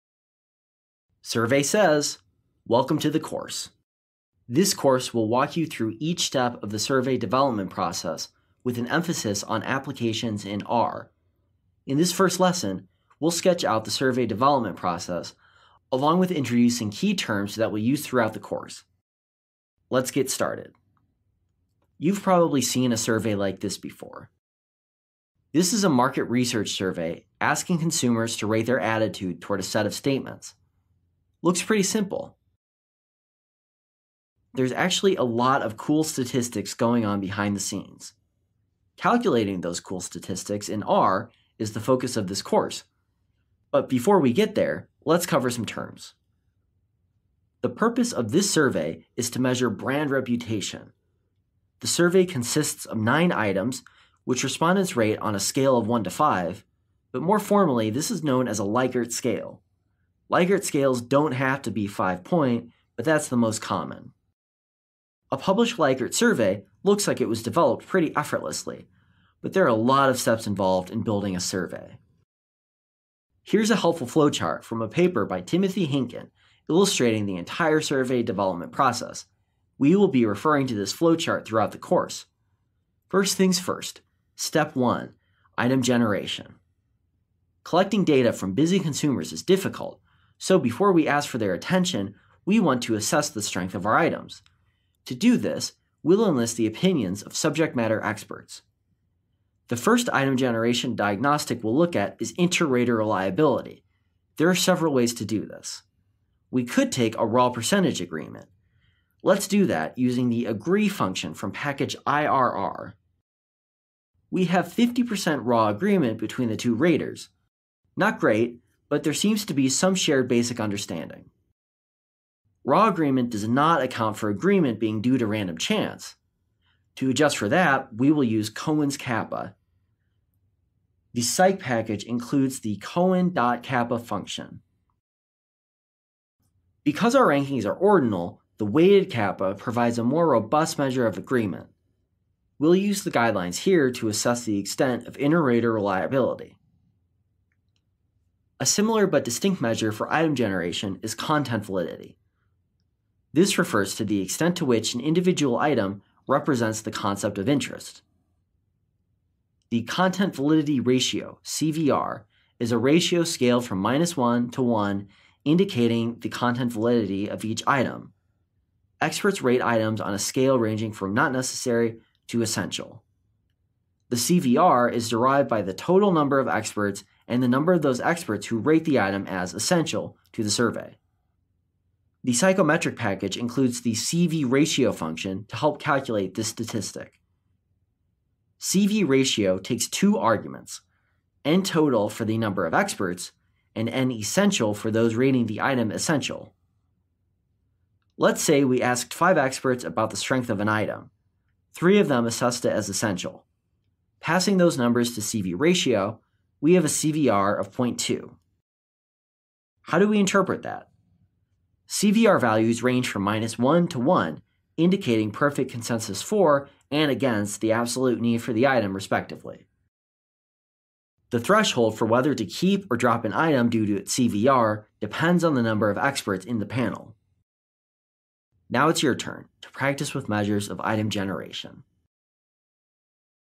R Tutorial: Surveys in marketing research
Want to learn more? at your own pace. More than a video, you'll learn hands-on coding & quickly apply skills to your daily work.

Survey says... welcome to the course.

This course will walk you through each step of the survey development process with an emphasis on applications in R. In this first lesson, we'll sketch out the survey development process, along with introducing key terms that we'll use throughout the course. Let's get started!

 You've probably seen a survey like this before.

This is a market research survey asking consumers to rate their attitude toward a set of statements. Looks pretty simple. There's actually a lot of cool statistics going on behind the scenes. Calculating those cool statistics in R is the focus of this course, but before we get there, let's cover some terms.

 The purpose of this survey is to measure brand reputation. The survey consists of nine items, which respondents rate on a "scale of one to five," but more formally this is known as a Likert scale. Likert scales don't have to be five-point, but that's the most common.

A published Likert survey looks like it was developed pretty effortlessly, but there are a lot of steps involved in building a survey.

 Here's a helpful flowchart from a paper by Timothy Hinkin illustrating the entire survey development process. We will be referring to this flowchart throughout the course.

First things first -- Step 1, item generation.

Collecting data from busy consumers is difficult, so before we ask for their attention we want to assess the strength of our items. To do this, we'll enlist the opinions of subject matter experts.

 The first item generation diagnostic we'll look at is inter-rater reliability. There are several ways to do this.

We could take a raw percentage agreement.

Let's do that using the agree() function from package i-r-r.

We have 50 percent raw agreement between the two raters -- not great, but there seems to be some basic shared understanding.

 Raw agreement does not account for agreement being due to random chance.

To adjust for that, we will use Cohen's kappa. The psych package includes the cohen-dot-kappa() function.

 Because our rankings are ordinal, the weighted kappa provides a more robust measure of agreement. We'll use the guidelines here to assess the extent of inter-rater reliability.

 A similar but distinct measure for item generation is content validity.

This refers to the extent to which an individual item represents the concept of interest.

The Content Validity Ratio (CVR) is a ratio scaled from -1 to 1 indicating the content validity of each item. Experts rate items on a scale ranging from "Not Necessary" to "Essential."

The CVR is derived by the total number of experts and the number of those experts who rate the item as "essential" to the survey.

 The psychometric package includes the CVratio() function to help calculate this statistic.

CVratio() takes two arguments: NTOTAL for the number of experts, and NESSENTIAL for the those rating the item essential.

Let's say we asked five experts about the strength of an item. Three of them assessed it as essential. Passing those numbers to CVratio() we have a CVR of point-two.

 How do we interpret that? CVR values range from -1 to 1, indicating perfect consensus for and against the absolute need for the item, respectively.

The threshold for whether to drop or keep an item due to its CVR depends on the number of experts in the panel.

 Now it's your turn to practice with measures of item generation.